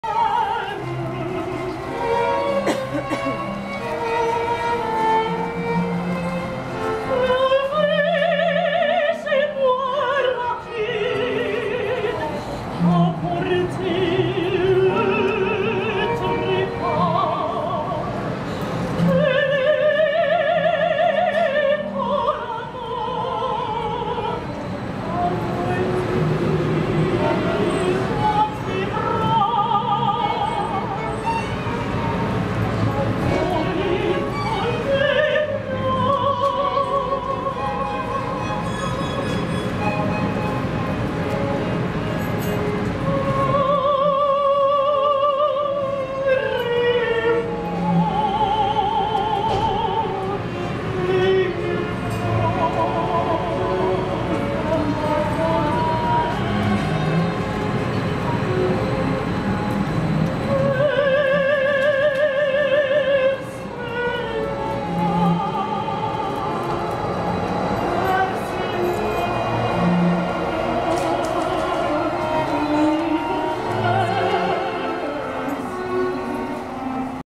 104 Underground
Mercury Opera takes over the Bay/Enterprise LRT Station in downtown Edmonton.